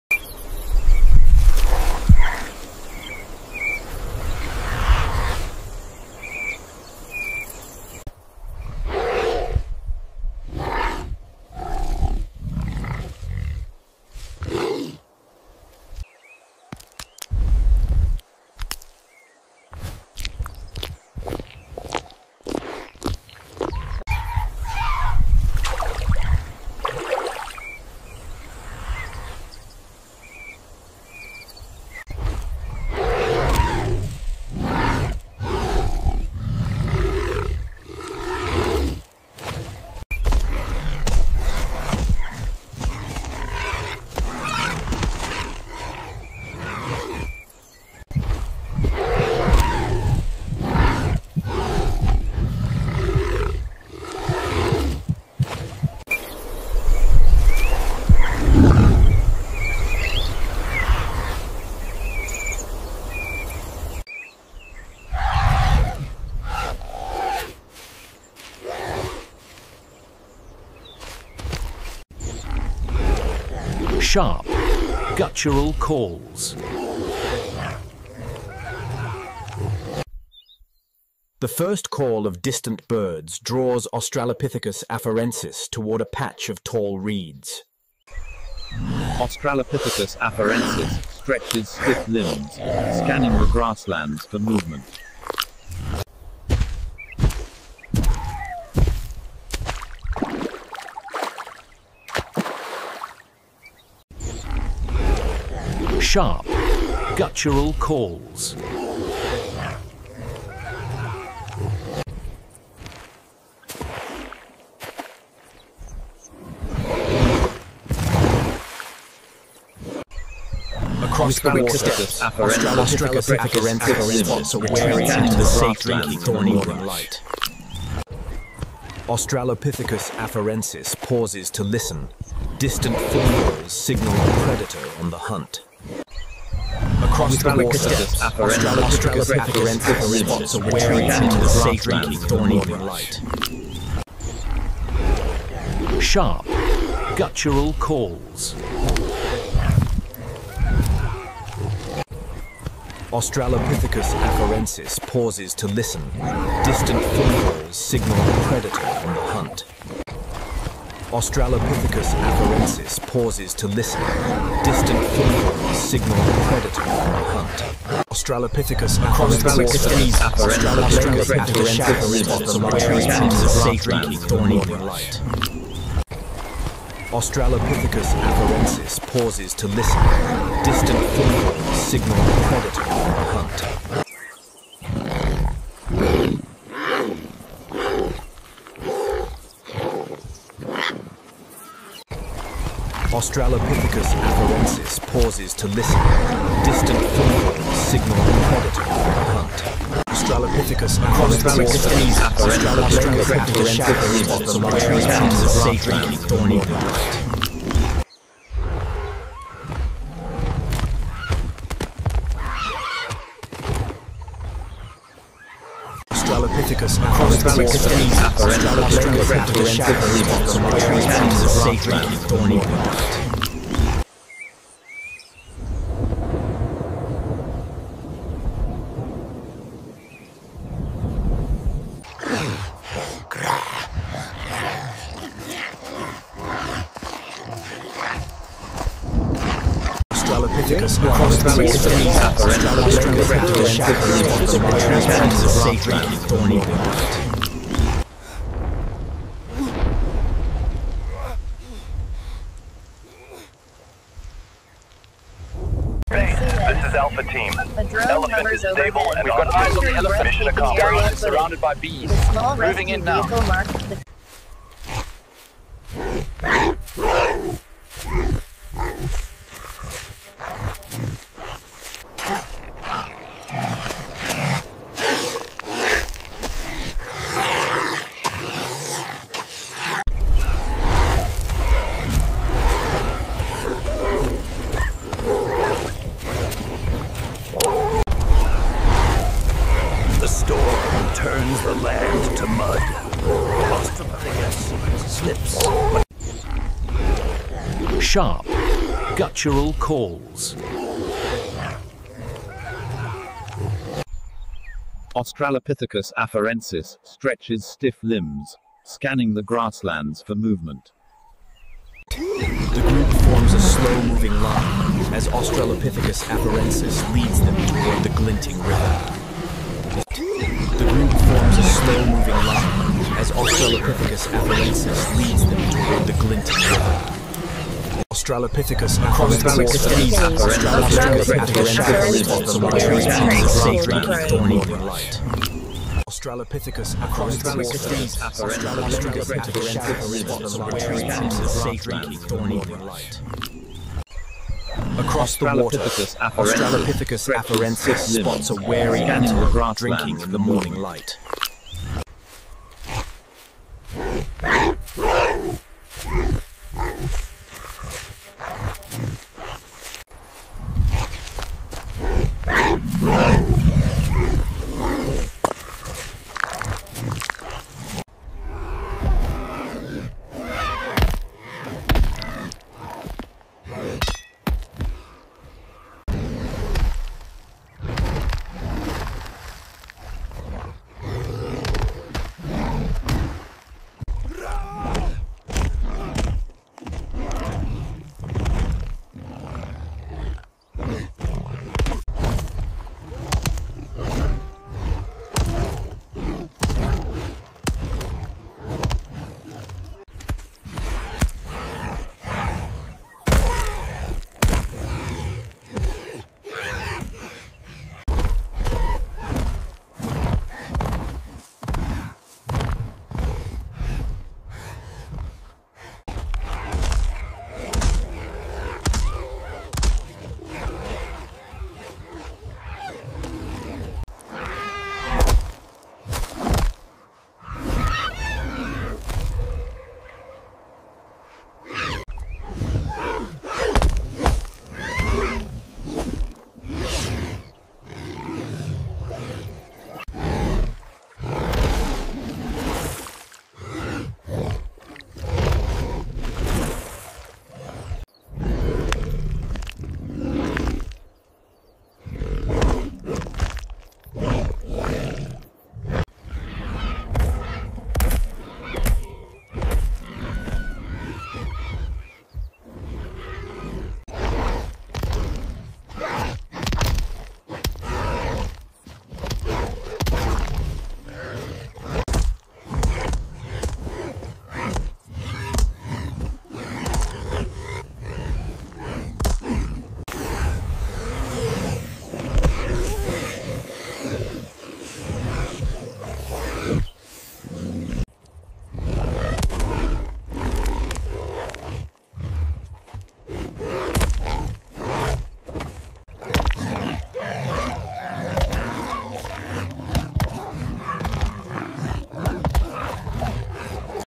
Life 3.4 Million Years Ago | How Did the First Humans Defend Their Young?
Travel back in time to 3.4 million years ago, when our earliest human ancestors roamed the African plains. This was a world filled with predators — saber-toothed cats, giant birds of prey, and other dangerous creatures. Survival wasn’t guaranteed, and protecting the young was one of the most important challenges early humans faced.

In this documentary-style video, we explore:
🪨 The daily life of Australopithecus afarensis and other early hominins
🛡️ How early humans defended their children without modern weapons
🤝 The role of family and group cooperation in survival
🔥 The use of natural tools, environmental awareness, and community protection strategies
🌍 How predator threats shaped the evolution of human behavior and social bonds

By understanding how our ancestors protected their young, we gain insight into the origins of human resilience, cooperation, and love for family — traits that still define us today.